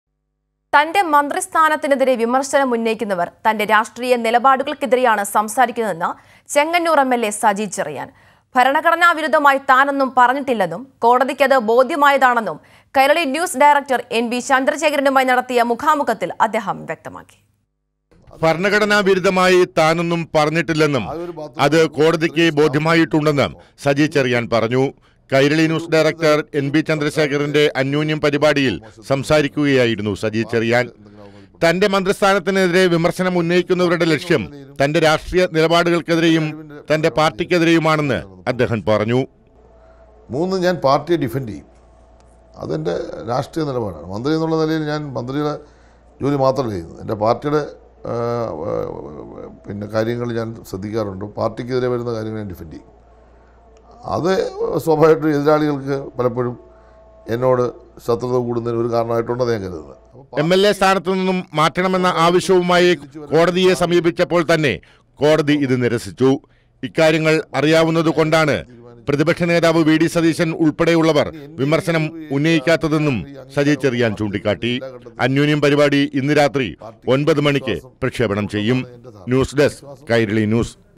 ഭരണഘടന വിരുദ്ധമായി താനൊന്നും പറഞ്ഞിട്ടില്ല; സജി ചെറിയാൻ | Kairali News
എന്റെ മന്ത്രിസ്ഥാനത്തിനെതിരെ വിമർശനം ഉന്നയിക്കുന്നവർ എന്റെ രാഷ്ട്രീയ നിലപാടുകൾക്കെതിരെയാണ് സംസാരിക്കുന്നത്